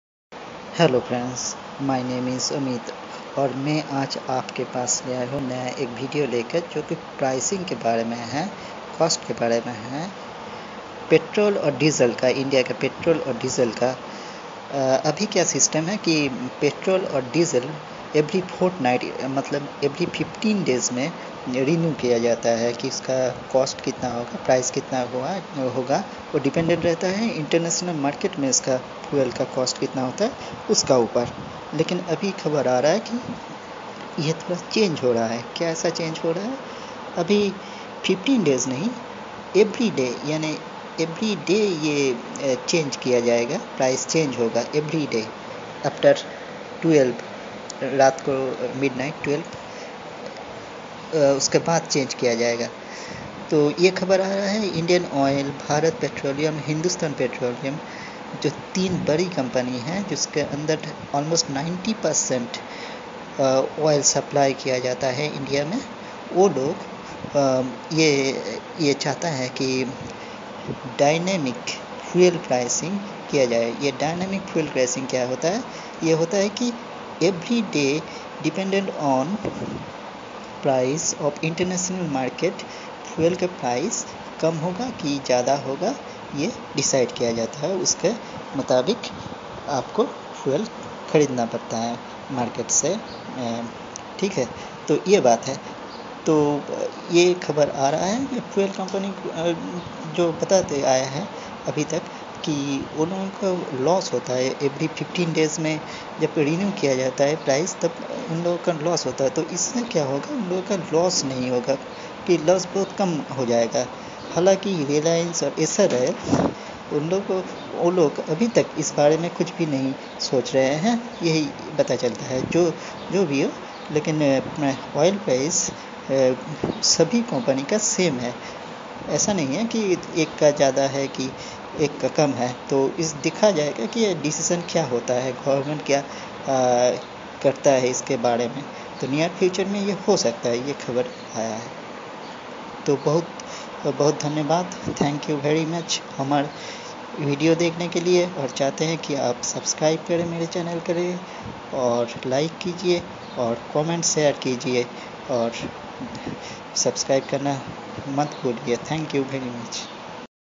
INDIAN OIL COMPANY KA NAYA DECITION KAYA HAI JANIYE VIDEO SE
INDIAN OIL COMPANY LOSS SE BACHNE KE LIYE NAYA KUCH KARNA CHATA HAI...